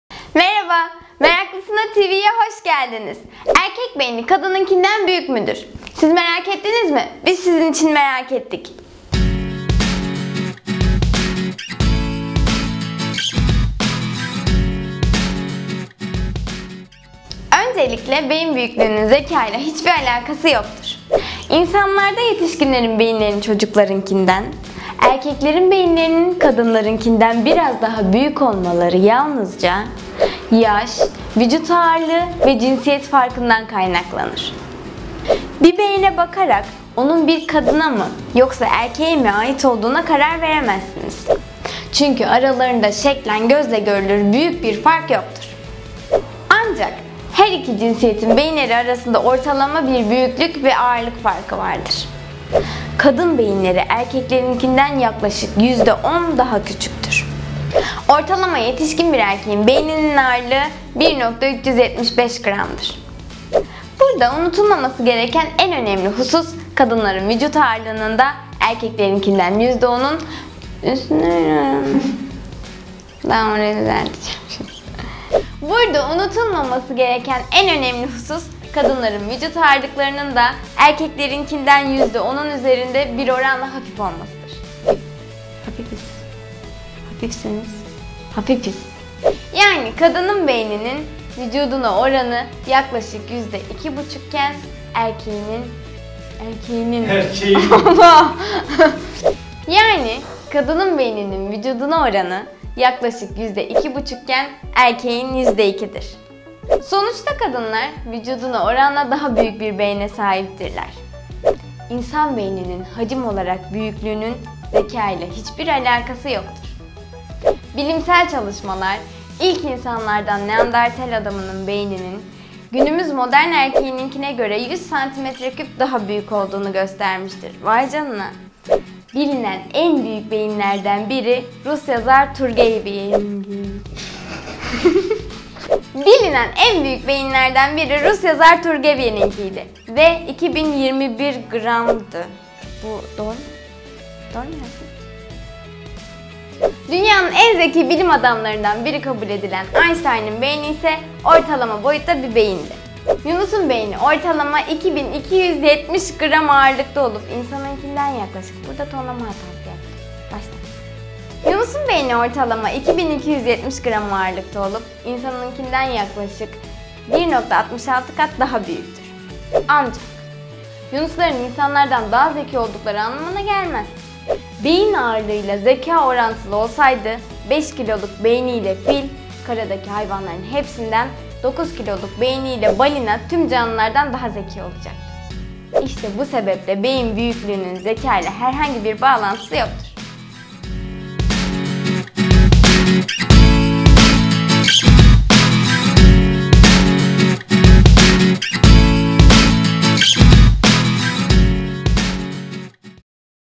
Erkek Beyni Kadın Beyninden Büyük müdür?
Erkek beyni kadın beyninden büyük müdür ? Hiç kendinize bunu sordunuz mu? Biz sorduk merak ettik ve öğrenip sizlere anlatmaya karar verdik İyi seyirler dileriz.
Meraklısına TV , Merak edilen ama üşengeçliğimizden araştırma yapmadığımız tüm bilgileri sizin yerinize araştırıyor.

Spiker                        : Tuğba Akgündüz
Genel Yayın              : Emre Kutlu
Genel Yayın Yrd.      : Ece Uğur
Görüntü Yönetmeni: Ferhat Onur Ceylan
Kurgu , Montaj         : Harun Demirel & Burak Demirbaş

Pixel Prodüksiyon katkılarıyla.